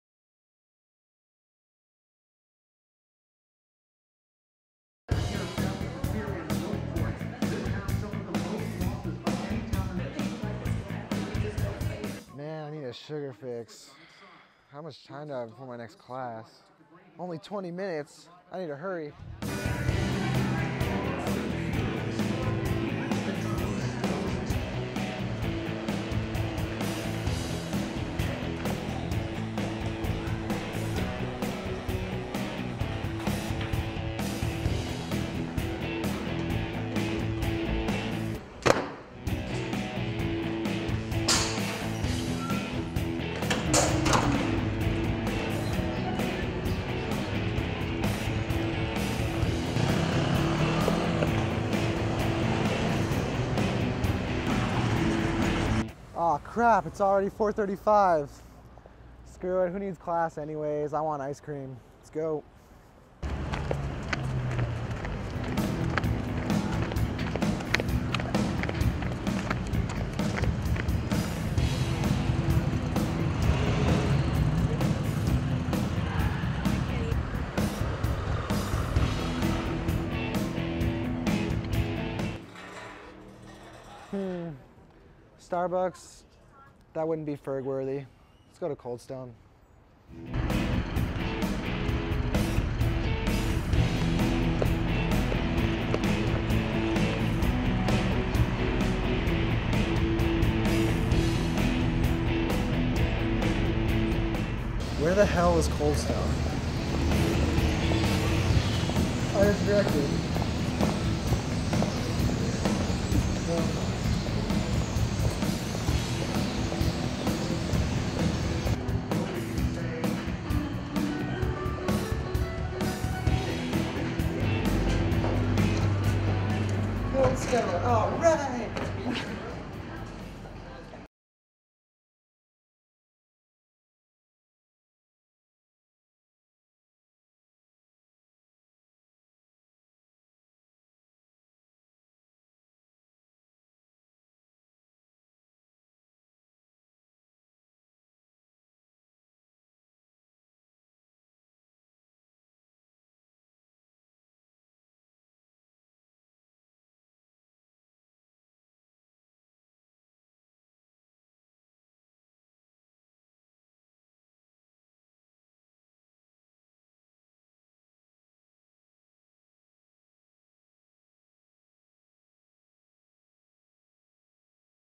Matching Action Mini Movie
A little comedic project a classmate and I did together demonstrating matching action in videography.